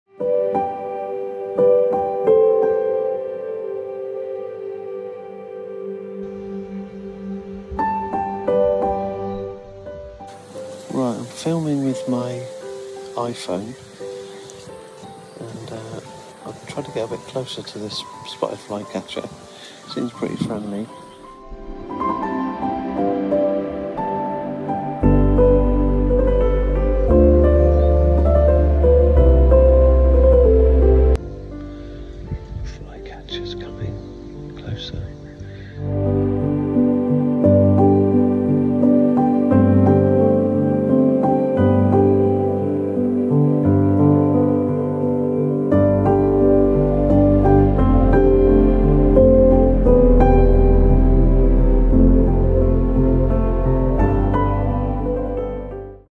Photographing Spotted Flycatchers in Lesvos
A field trip to photograph the Spotted Flycatcher in Lesvos, using the Sony A1